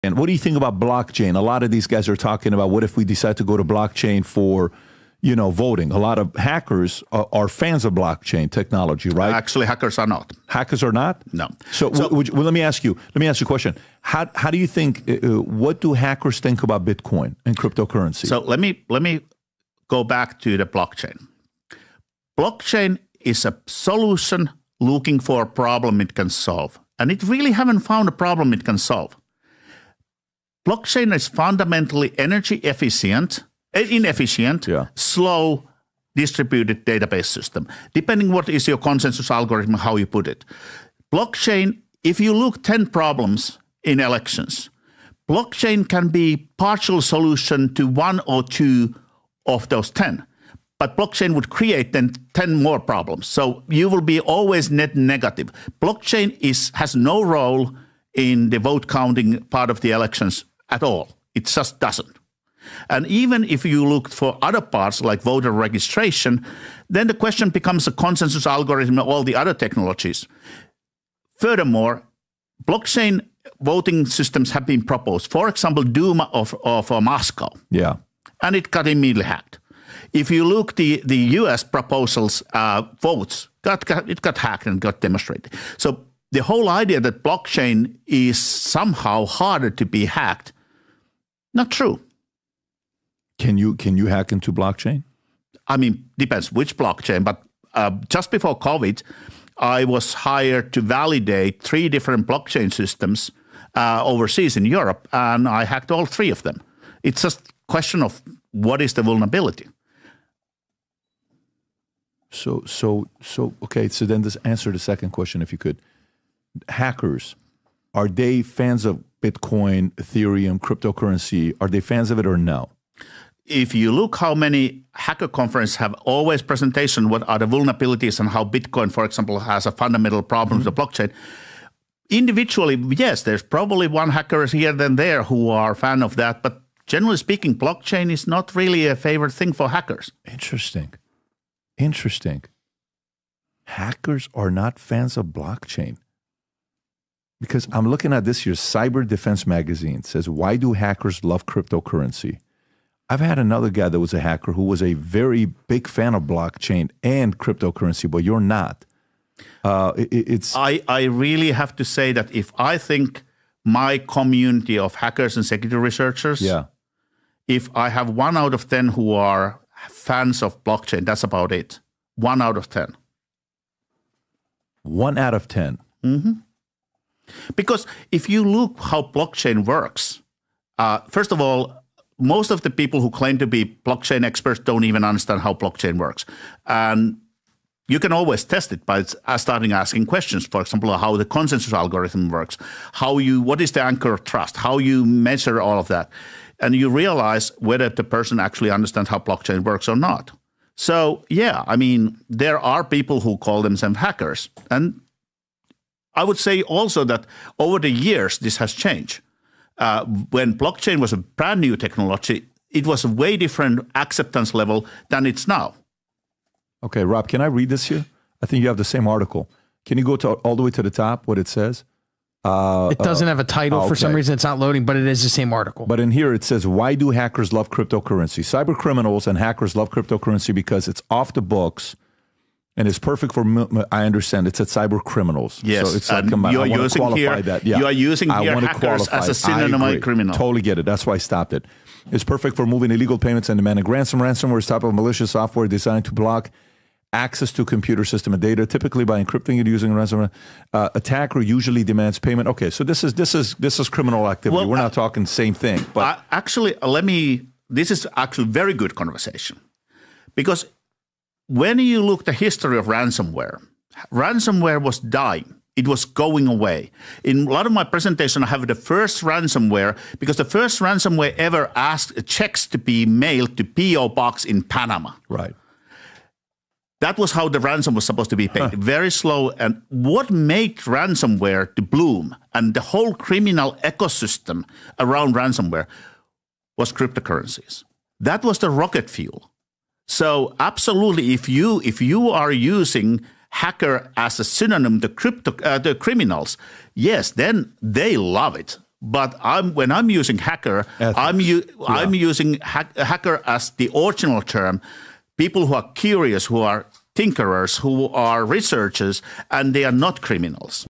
Harri Hursti EXPOSES Blockchain's Hidden Security Weaknesses
Blockchain and cryptocurrency are often touted as secure and revolutionary, but is that really true? In this video, hacker and cybersecurity expert Harri Hursti dives deep into the vulnerabilities of blockchain technology and exposes the flaws that many don’t talk about. Discover how blockchain can be hacked, what crypto security flaws exist, and how these vulnerabilities could impact your investments. Harri Hursti’s expert insights will change the way you think about blockchain security and the risks in the world of cryptocurrency.